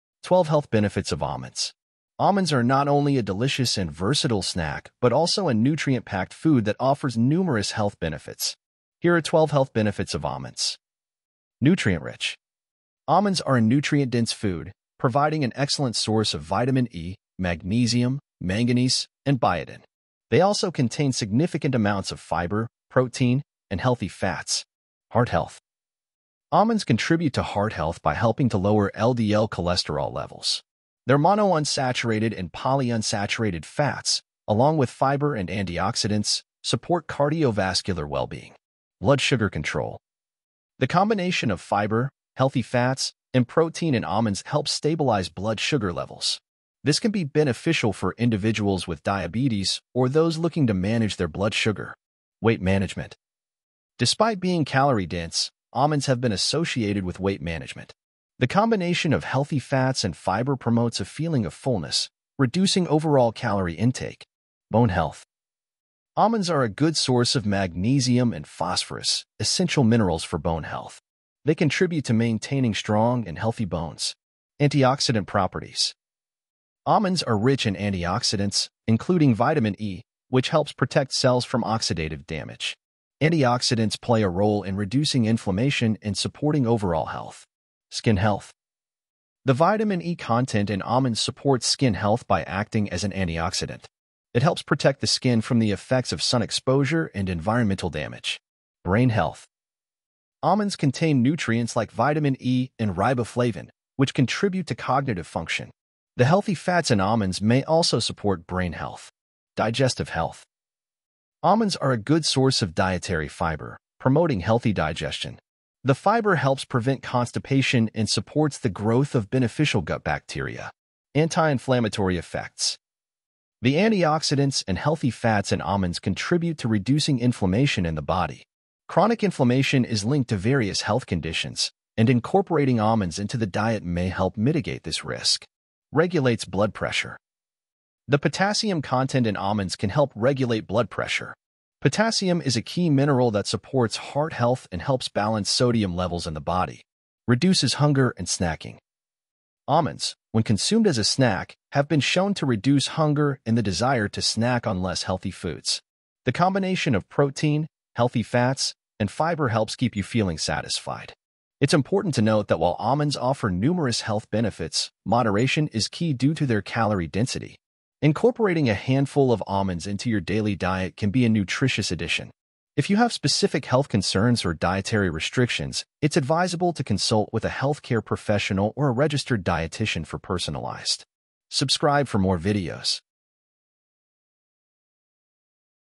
12 HEALTH BENEFITS OF ALMONDS
Almonds are not only a delicious and versatile snack but also a nutrient-packed food that offers numerous health benefits.
Here are 12 health benefits of almonds:

Nutrient-Rich:
Almonds are a nutrient-dense food, providing an excellent source of vitamin E, magnesium, manganese, and biotin. They also contain significant amounts of fiber, protein, and healthy fats.

Heart Health:
Almonds contribute to heart health by helping to lower LDL cholesterol levels. Their monounsaturated and polyunsaturated fats, along with fiber and antioxidants, support cardiovascular well-being.

Blood Sugar Control:
The combination of fiber, healthy fats, and protein in almonds helps stabilize blood sugar levels. This can be beneficial for individuals with diabetes or those looking to manage their blood sugar.

Weight Management:
Despite being calorie-dense, almonds have been associated with weight management. The combination of healthy fats and fiber promotes a feeling of fullness, reducing overall calorie intake.

Bone Health:
Almonds are a good source of magnesium and phosphorus, essential minerals for bone health. They contribute to maintaining strong and healthy bones.

Antioxidant Properties:
Almonds are rich in antioxidants, including vitamin E, which helps protect cells from oxidative damage. Antioxidants play a role in reducing inflammation and supporting overall health.

Skin Health:
The vitamin E content in almonds supports skin health by acting as an antioxidant. It helps protect the skin from the effects of sun exposure and environmental damage.

Brain Health:
Almonds contain nutrients like vitamin E and riboflavin, which contribute to cognitive function. The healthy fats in almonds may also support brain health.

Digestive Health:
Almonds are a good source of dietary fibre, promoting healthy digestion. The fibre helps prevent constipation and supports the growth of beneficial gut bacteria.

Anti-Inflammatory Effects:
The antioxidants and healthy fats in almonds contribute to reducing inflammation in the body. Chronic inflammation is linked to various health conditions, and incorporating almonds into the diet may help mitigate this risk.

Regulates Blood Pressure:
The potassium content in almonds can help regulate blood pressure. Potassium is a key mineral that supports heart health and helps balance sodium levels in the body.

Reduces Hunger and Snacking:
Almonds, when consumed as a snack, have been shown to reduce hunger and the desire to snack on less healthy foods. The combination of protein, healthy fats, and fibre helps keep you feeling satisfied.

It's important to note that while almonds offer numerous health benefits, moderation is key due to their calorie density. Incorporating a handful of almonds into your daily diet can be a nutritious addition. If you have specific health concerns or dietary restrictions, it's advisable to consult with a healthcare professional or a registered dietitian for personalized.

👍Kirkland Signature Daily Multi, 500

 ⭐26 Key Multivitamins & Minerals for Women & Men.